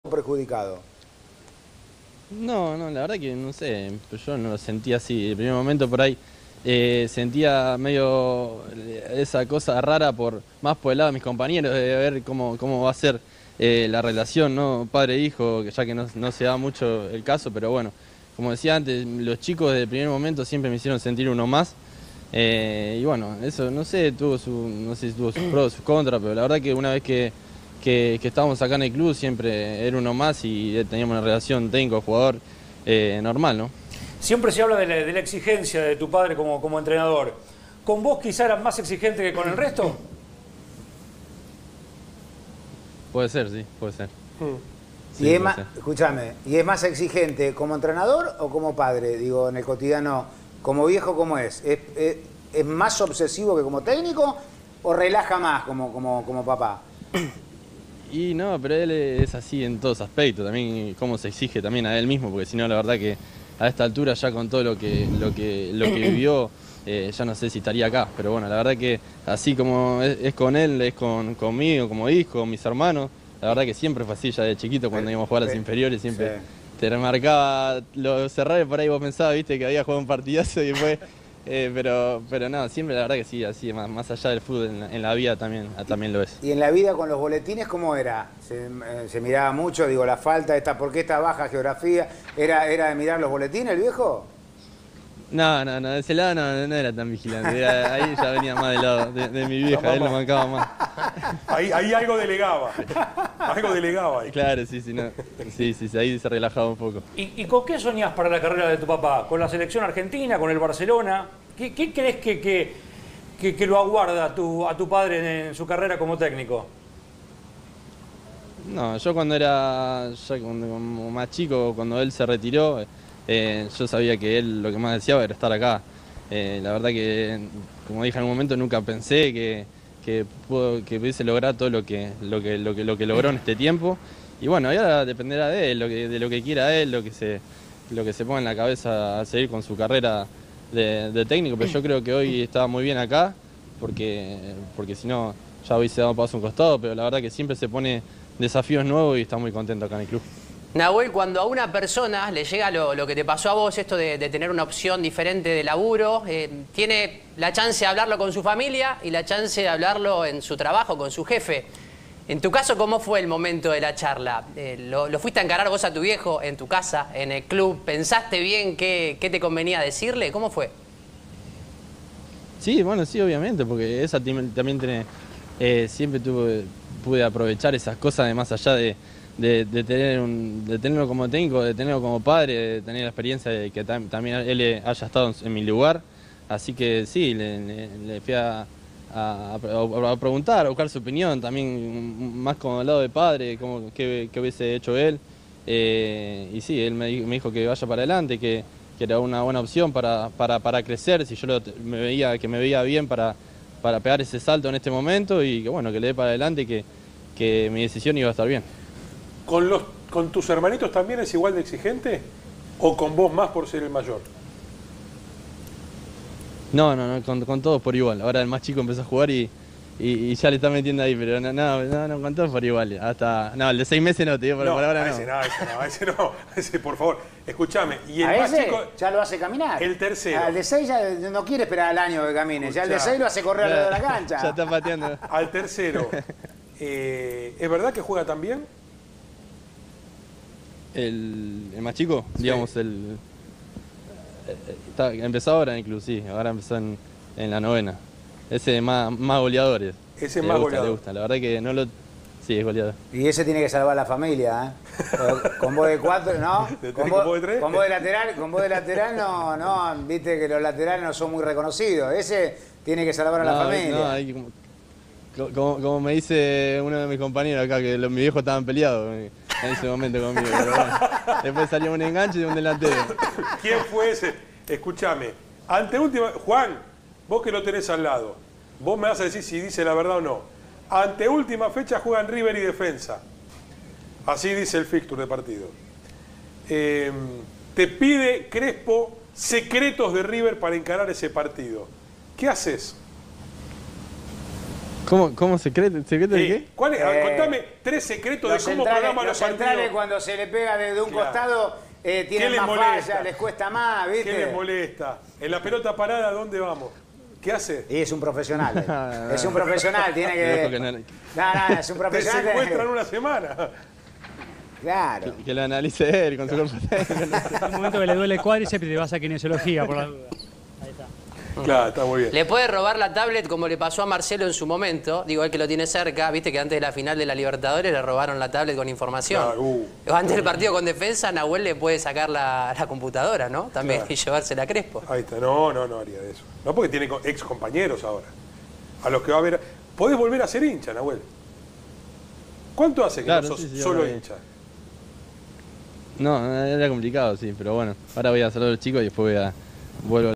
Nahuel Gallardo, la relación con su padre y su salida de River
El lateral, hijo del Muñeco, se mostró feliz por sumarse a Defensa y Justicia. "Me dijo que vaya para adelante, que me veía bien para pegar el salto", afirmó.